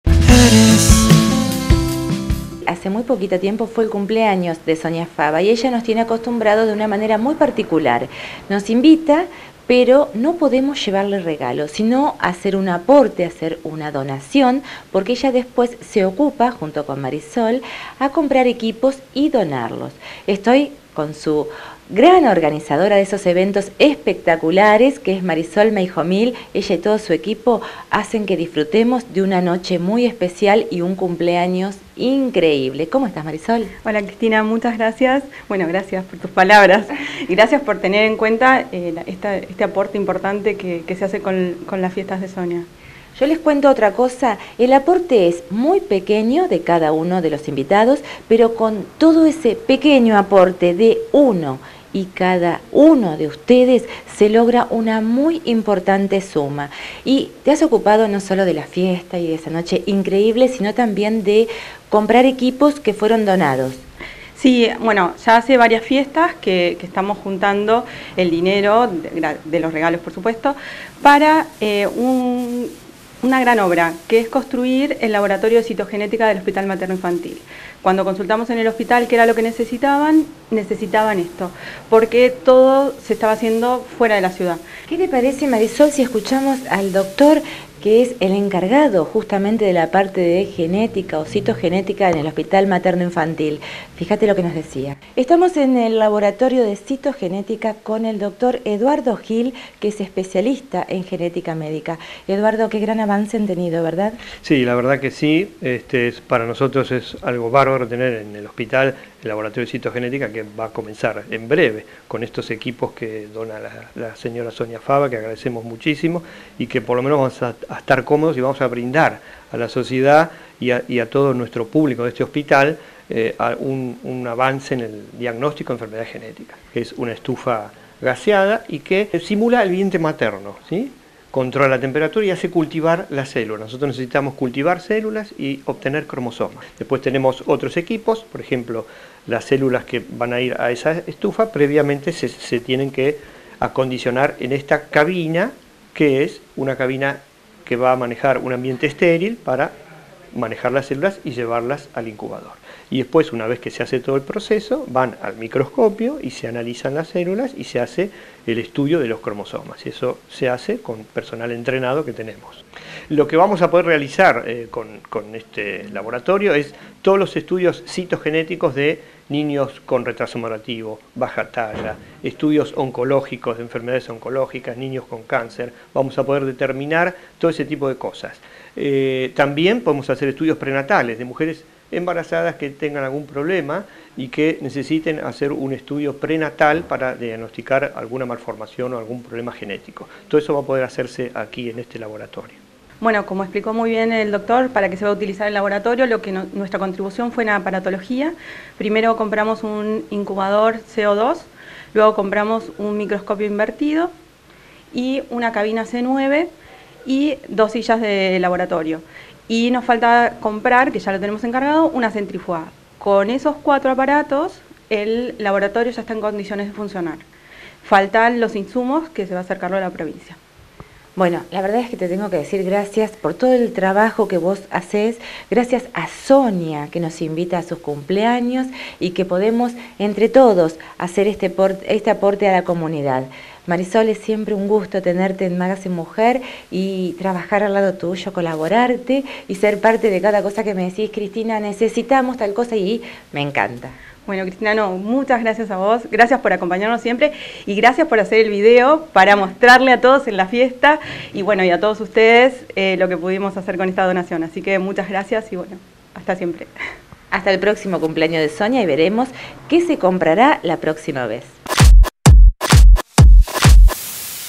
Nuevo laboratorio de citogenética en Mar del Plata.
Entrevista a Marisol Meijomil.